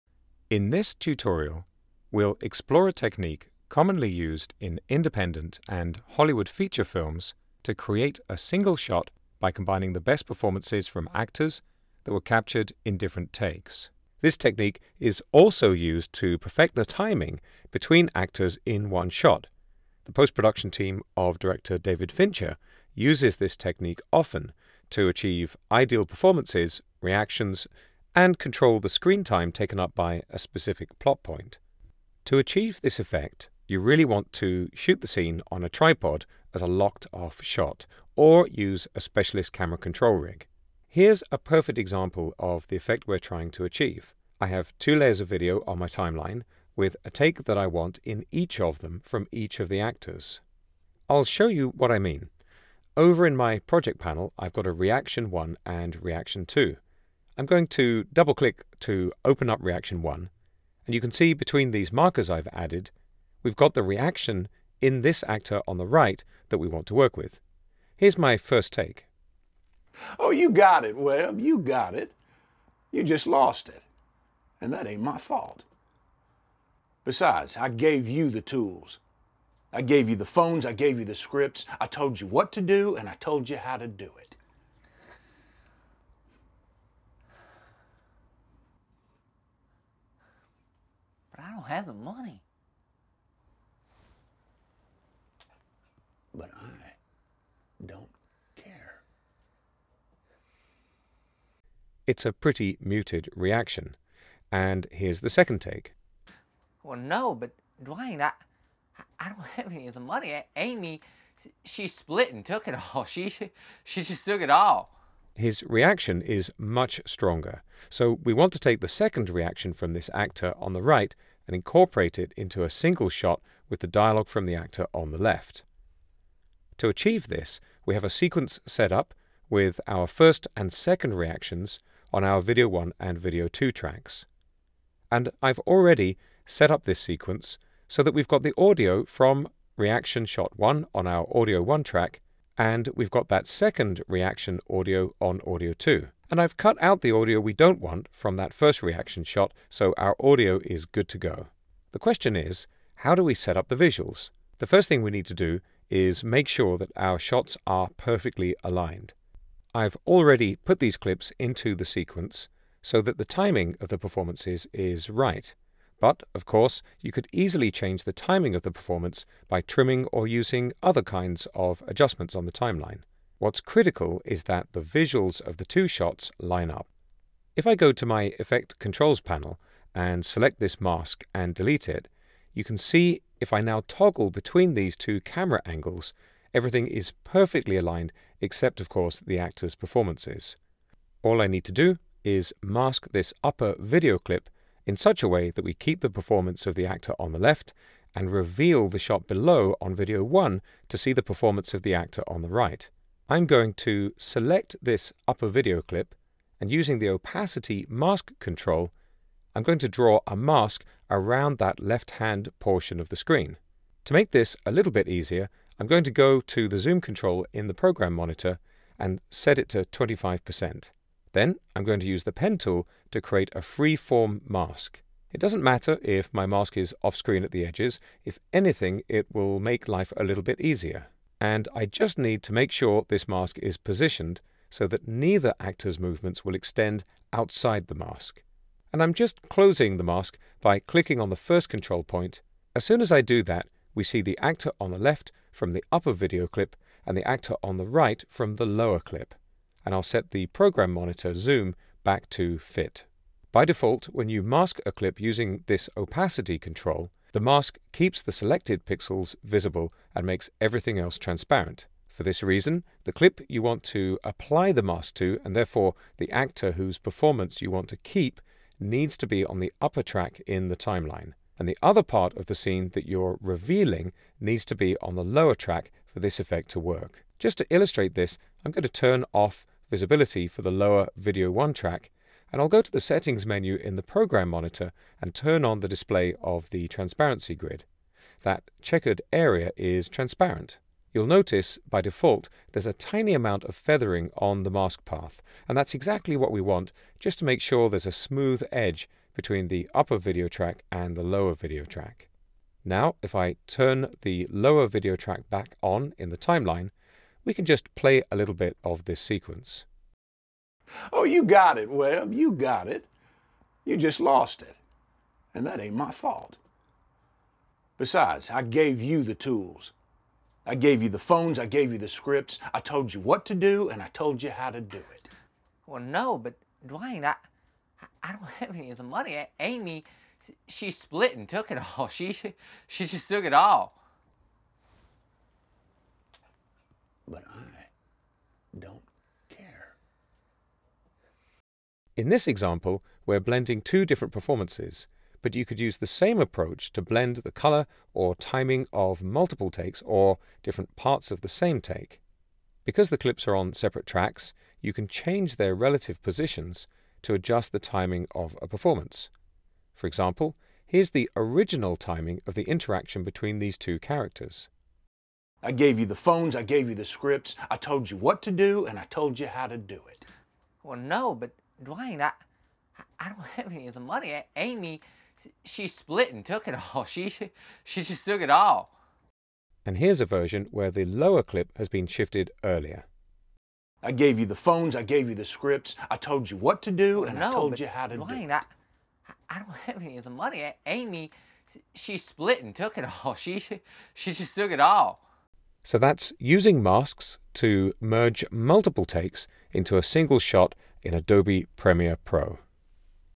Use Basic Compositing to Combine your Best Clips in Premiere Pro | Adobe Creative Cloud
Combine two takes of a scene in Adobe Premiere Pro CC to get the best performance from your actors.